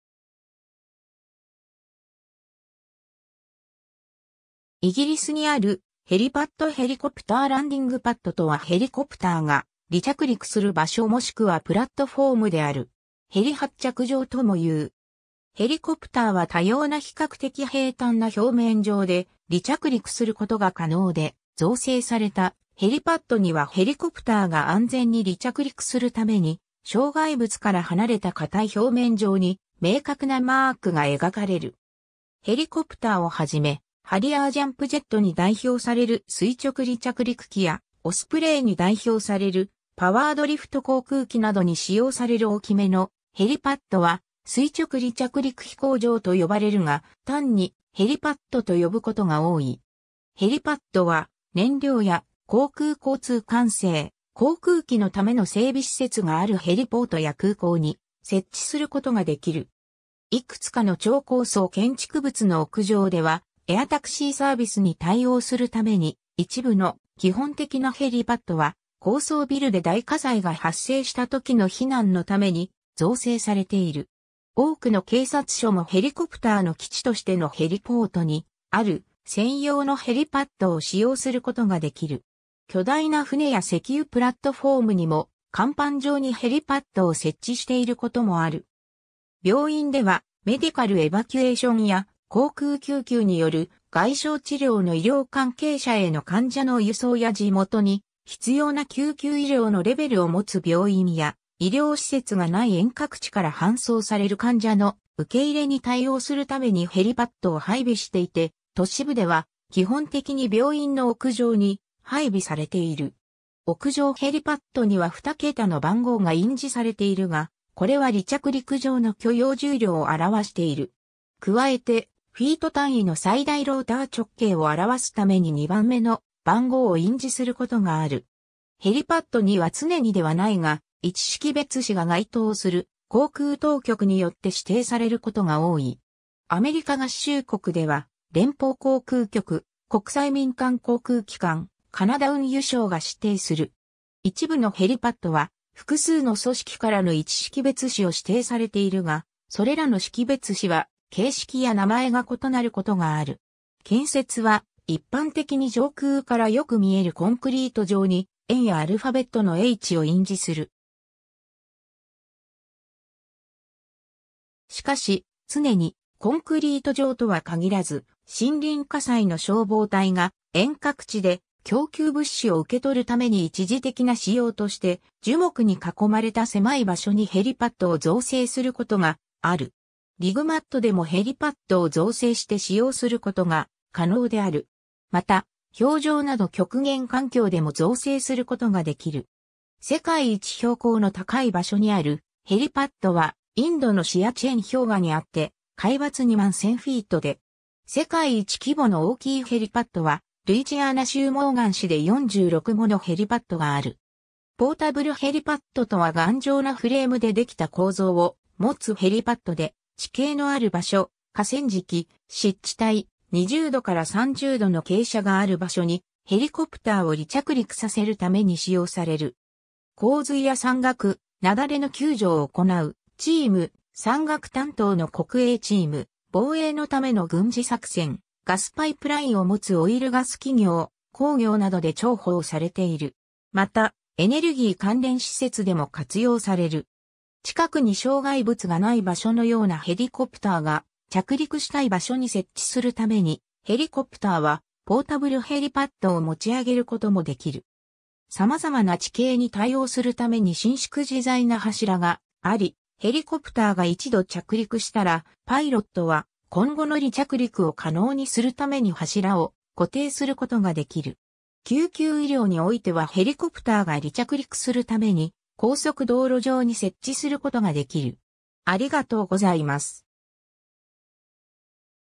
ヘリパッド
イギリスにあるヘリパッド ヘリコプターランディングパッド（ヘリパッド、英語: helipad）とはヘリコプターが離着陸する場所もしくはプラットフォームである。
ヘリ発着場 (ヘリはっちゃくじょう) ともいう。
ヘリコプターは多様な比較的平坦な表面上で離着陸することが可能で、造成されたヘリパッドにはヘリコプターが安全に離着陸するために障害物から離れた硬い表面上に明確なマークが描かれる。
 ヘリコプターを始め、ハリアー・ジャンプ・ジェットに代表される垂直離着陸機（VTOL）や、オスプレイに代表されるパワード・リフト航空機などに使用される大きめのヘリパッドは、垂直離着陸飛行場（vertiport）と呼ばれるが、単にヘリパッドと呼ぶことが多い。
 ヘリパッドは燃料や航空交通管制、航空機のための整備施設があるヘリポートや空港に設置することができる。
 いくつかの超高層建築物の屋上ではエアタクシーサービスに対応するために、一部の基本的なヘリパッドは高層ビルで大火災が発生した時の避難のために造成されている。
多くの警察署もヘリコプター (en) の基地としてのヘリポートにある専用のヘリパッドを使用することができる。
巨大な船や石油プラットフォームにも甲板上にヘリパッドを設置していることもある（一般的にヘリコプターデッキもしくはヘリデッキと称される）。
 病院では、メディカル・エヴァキュエーションや航空救急による外傷治療の医療関係者への患者の輸送や地元に必要な救急医療のレベルを持つ病院や医療施設が無い遠隔地から搬送される患者の受け入れに対応するためにヘリパッドを配備していて、都市部では基本的に病院の屋上に配備されている。
 屋上ヘリパッドには2桁の番号が印字されているが、これは離着陸場の許容重量（千ポンド単位）を表している。
加えて、フィート単位の最大ローター直径を表すために2番目の番号を印字することがある。
 ヘリパッドには常にではないが位置識別子が該当する航空当局によって指定されることが多い。
アメリカ合衆国では連邦航空局、国際民間航空機関、カナダ運輸省が指定する。
一部のヘリパッドは複数の組織からの位置識別子を指定されているが、それらの識別子は形式や名前が異なることがある。
 建設は一般的に上空からよく見えるコンクリート上に円やアルファベットのHを印字する。
しかし、常にコンクリート上とは限らず、森林火災の消防隊が遠隔地で供給物資を受け取るために一時的な使用として樹木に囲まれた狭い場所にヘリパッドを造成することがある。
リグマットでもヘリパッドを造成して使用することが可能である。
また、氷上など極限環境でも造成することができる。
 世界一標高の高い場所にあるヘリパッドはインドのシアチェン氷河にあって海抜21,000フィート（6,400メートル）で。
世界一規模の大きいヘリパッドはルイジアナ州モーガン市で46ものヘリパッドがある。
 ポータブルヘリパッドとは頑丈なフレームでできた構造を持つヘリパッドで、地形のある場所、河川敷、湿地帯、20度から30度の傾斜がある場所にヘリコプターを離着陸させるために使用される。
洪水や山岳、雪崩の救助を行うチーム、山岳担当の国営チーム、防衛のための軍事作戦、ガスパイプラインを持つオイル・ガス企業、鉱業などで重宝されている。
また、エネルギー関連施設（主に地形のある場所にある風車）でも活用される。
近くに障害物がない場所のようなヘリコプターが着陸したい場所に設置するためにヘリコプターはポータブルヘリパッドを持ち上げることもできる。
様々な地形に対応するために伸縮自在な柱があり、ヘリコプターが一度着陸したらパイロットは今後の離着陸を可能にするために柱を固定することができる。
救急医療においてはヘリコプターが離着陸するために高速道路上に設置することができる。